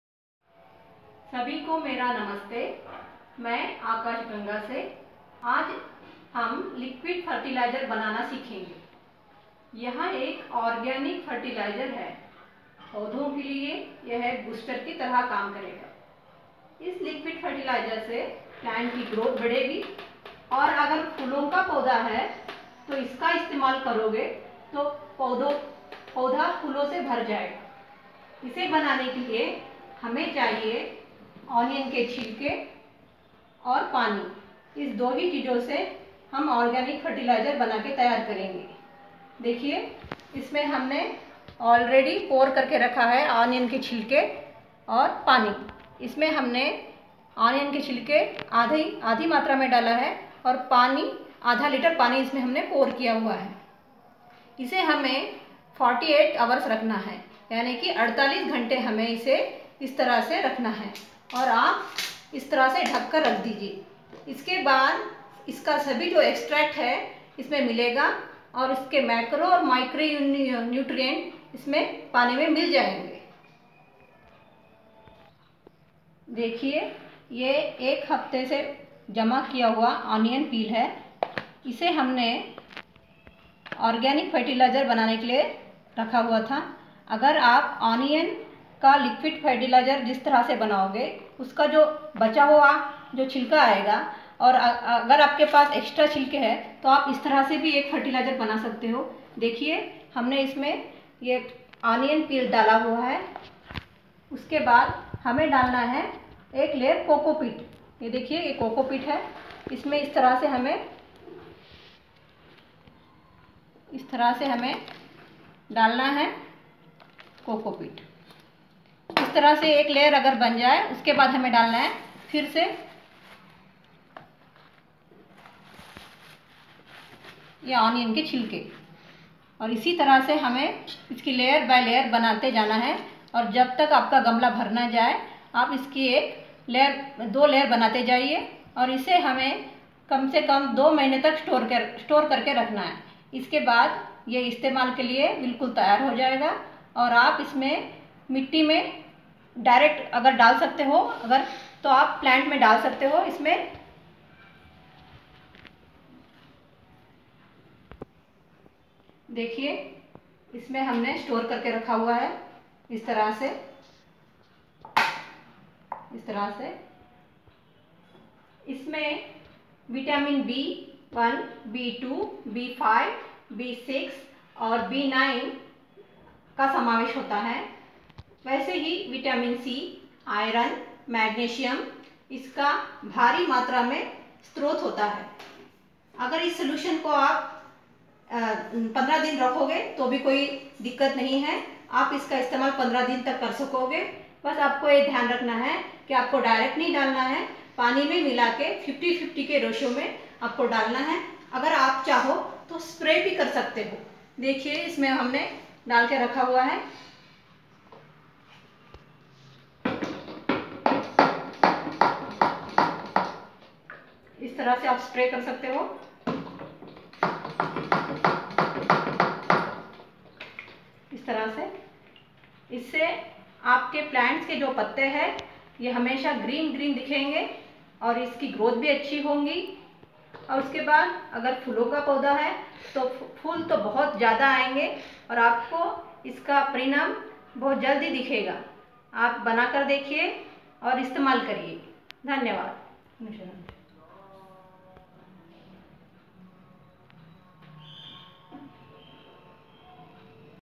Onion Peel NPK Fertiliser/ Organic Zero Budget Strong Compost And Liquid
Required Things to Make Liquid Fertiliser
1 onion peel
2 water